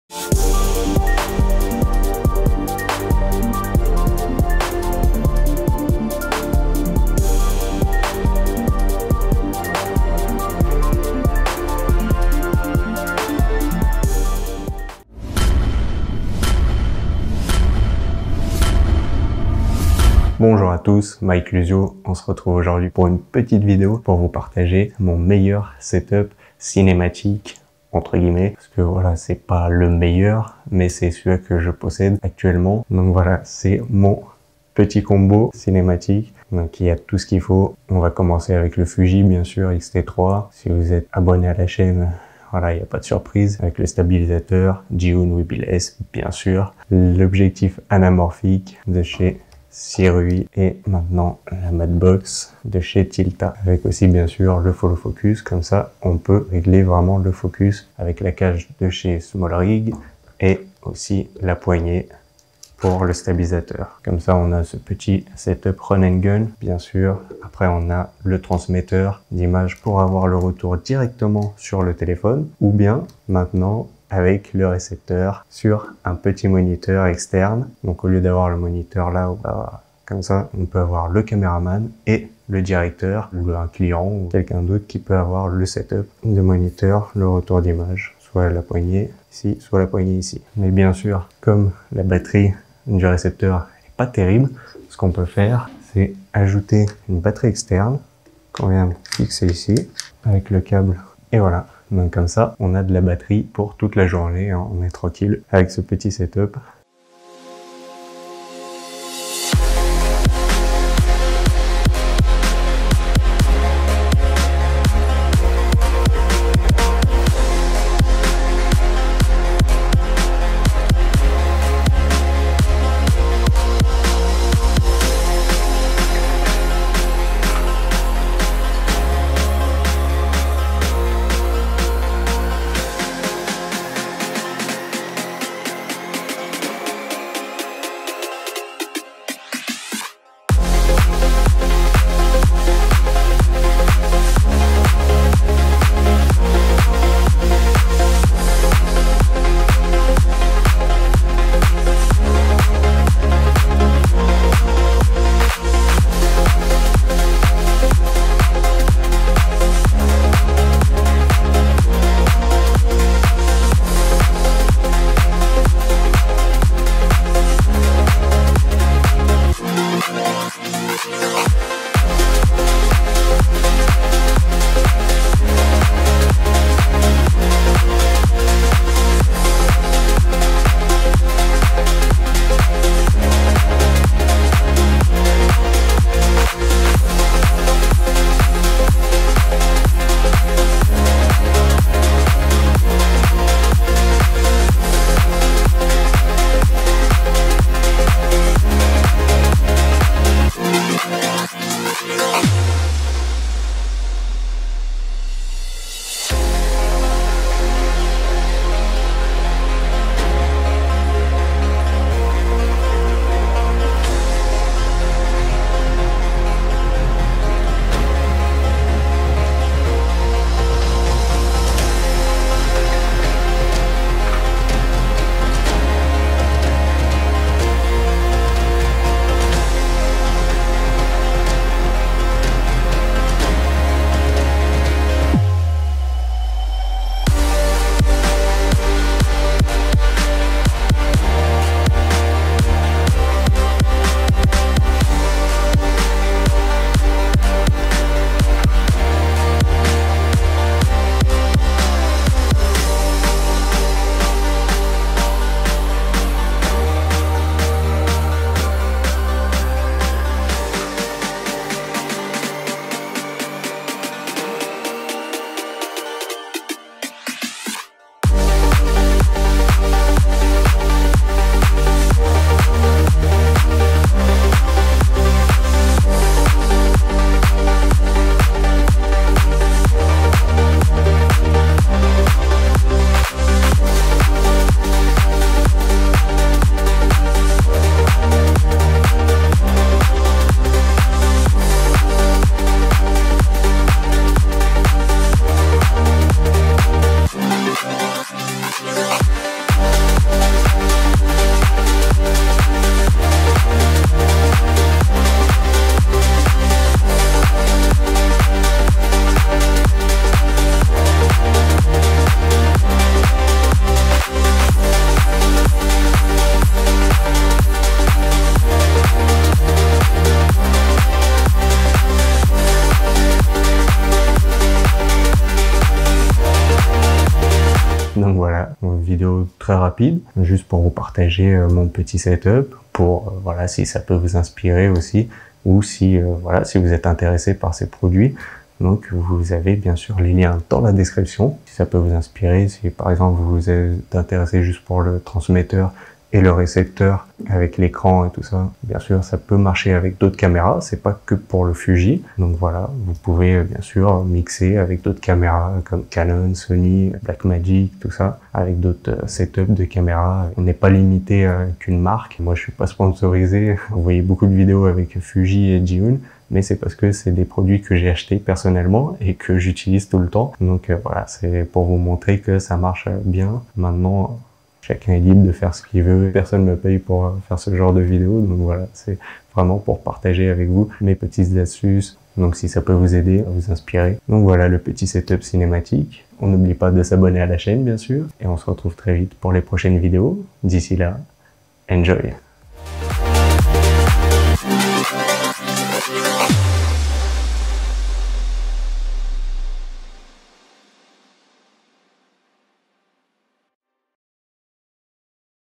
BEST DREAM CINEMATIC HEAVY GIMBAL COMBO RIG - FUJI X-T3 / ZHIYUN WEEBILL S / SIRUI ANAMORPHIC ( 4K )
Présentation de mon meilleur combo cinématique avec le FUJIFILM X-T3 sur le gimbal ZHIYUN WEEBILL S et ses accessoires ( le transMount image transmission transmetteur et récepteur ) l'écran FEELWORLD F5 et l'objectif SIRUI anamorphic 50mm 1.33x F1.8.